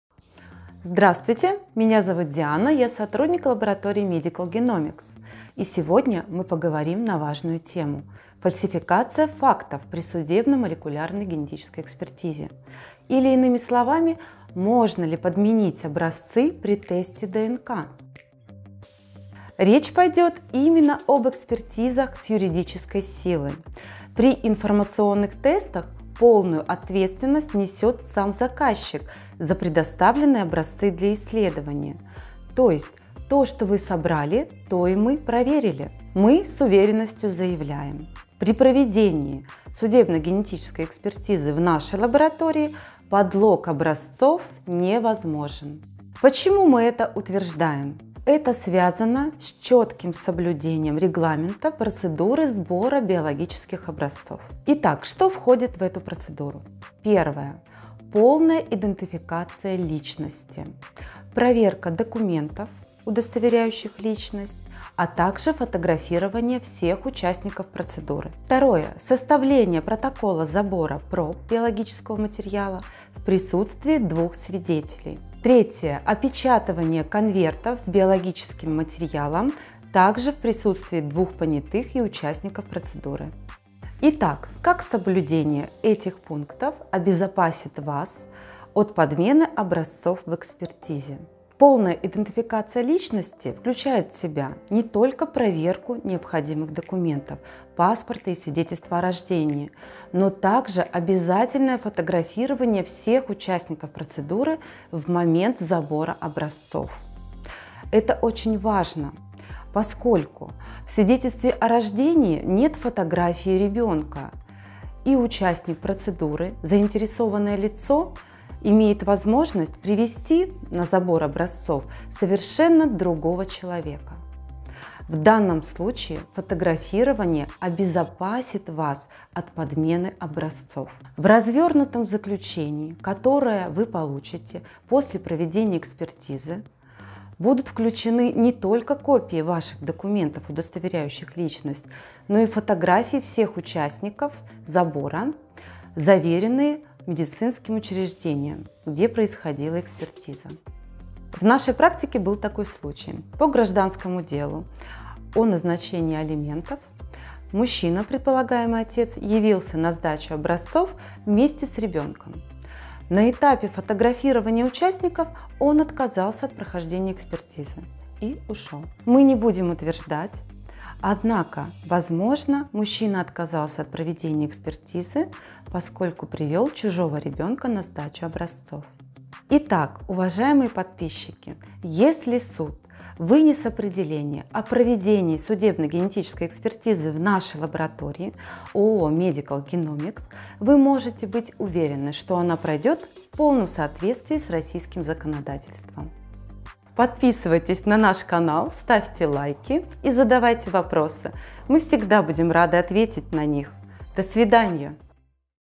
Тест ДНК на отцовство. Подмена образцов ребенка
В нашей лаборатории процедура забора биологического материала строго регламентирована, поэтому подмена образцов ребенка невозможна!
Смотрите видео и узнаете в чем наш секрет.